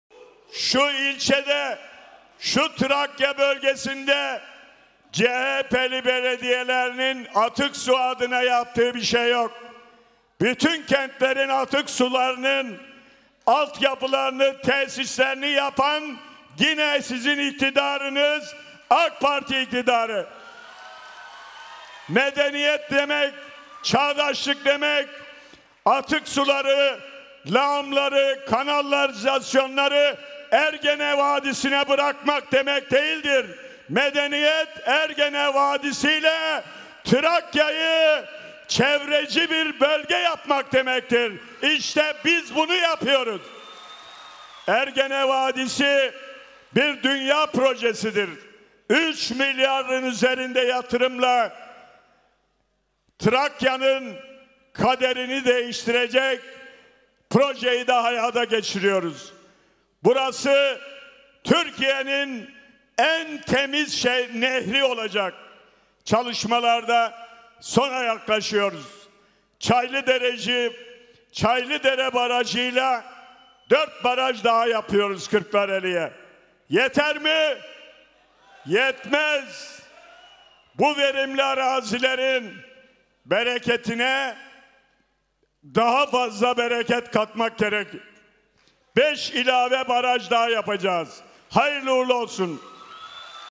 Başbakanımızdan atık su çalışmalarıyla ilgili açıklama
Başbakanımız: "Medeniyet demek,  çağdaşlık demek, atık suları, lağımları, kanalizasyonları Ergene Vadisi'ne bırakmak demek değildir, medeniyet Ergene Vadisiyle, Trakya'yı çevreci bir bölge yapmak demektir, işte biz bunu yapıyoruz."